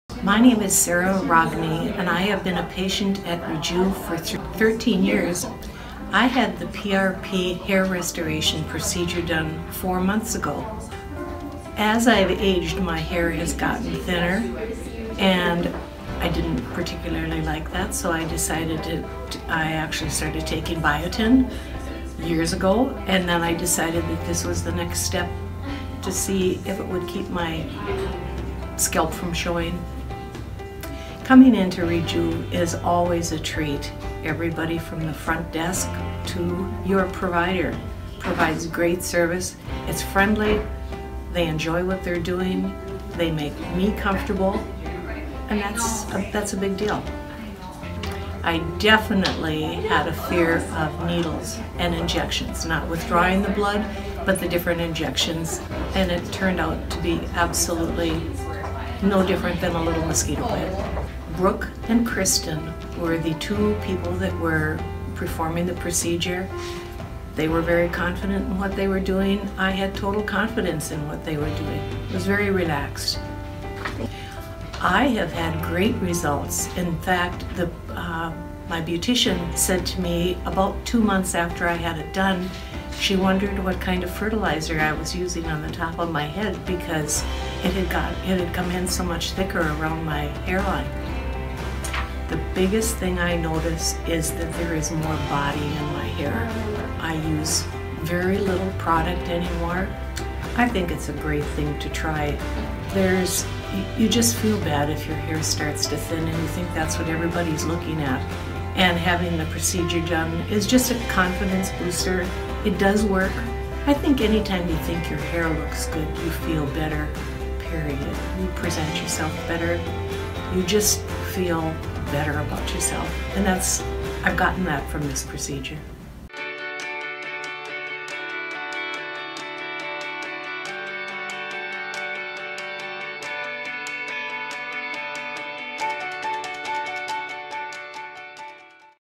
Real Patient Testimonials - PRP Hair Restoration - Rejuv Fargo
PRP Hair Restoration at Rejuv in Fargo ND -

💆Treatment: PRP Hair Restoration + Acell Technology
💁Nickname: Mane Grower
🎯Purpose: Reduce hair loss, slow down the process of hair loss and improve the thickness and appearance of the hair.
🛠How it works: Blood is drawn from the patient and is separated through a spinning process in a centrifuge. The PRP is harvested and then injected into the scalp.
🎉What you can expect: PRP when injected into the scalp acts like a fertilizer for the hair. Patients can expect stronger, thicker and of course MORE hair.
📌Notes: PRP can only be used on the patient it is harvested from.
🕓Time it takes: The treatment itself is fairly quick! But we will have you here about 1 hour.
💉Pain level: Minimal - Most patients consider this procedure tolerable.
🚧Recovery: No downtime
🏁Results: Patients can expect to see results in as little as 8 weeks
💵Cost: $$$
🤓 Facebook: Rejuv Medical Aesthetic Clinic